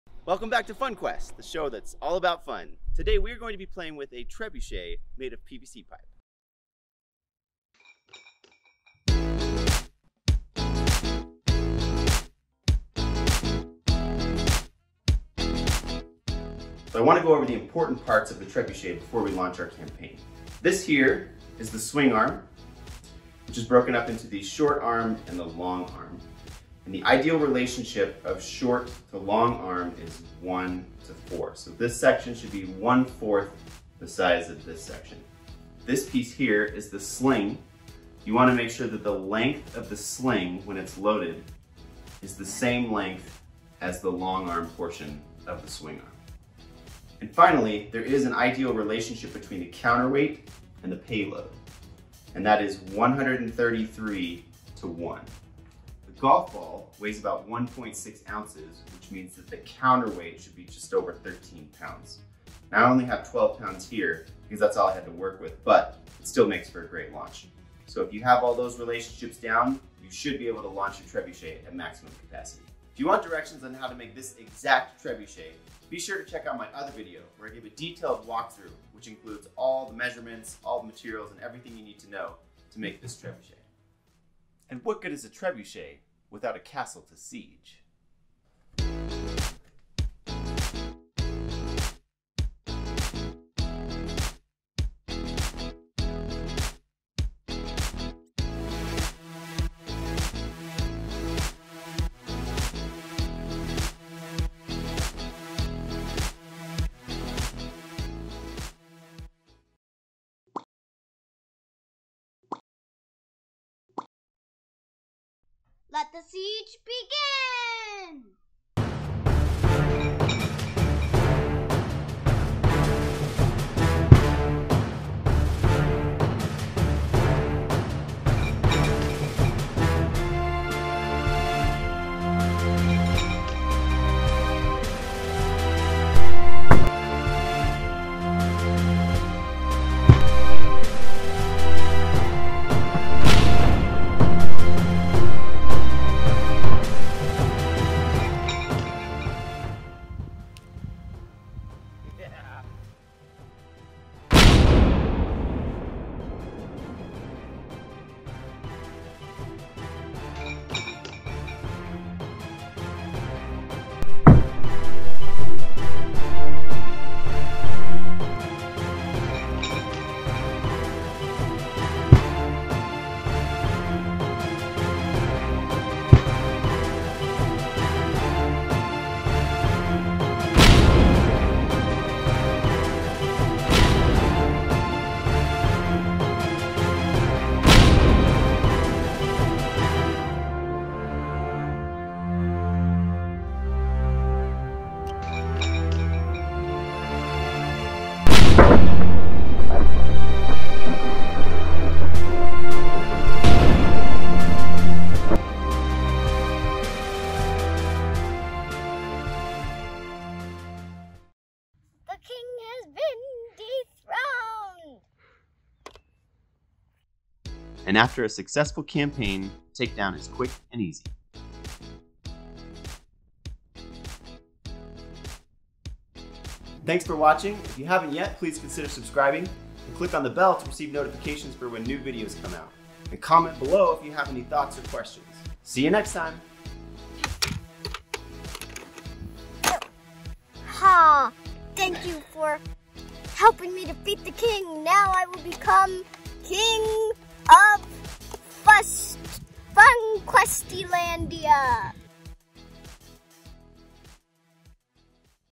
Demolishing a Lego Castle with a PVC Trebuchet
I begin with a quick demonstration of the trebuchet. After which, I go over the basics of a trebuchet, then build a Lego castle. Once the castle is complete, we begin the siege, launching tons of golf balls. Will the castle withstand the the siege? Will the mini figures survive?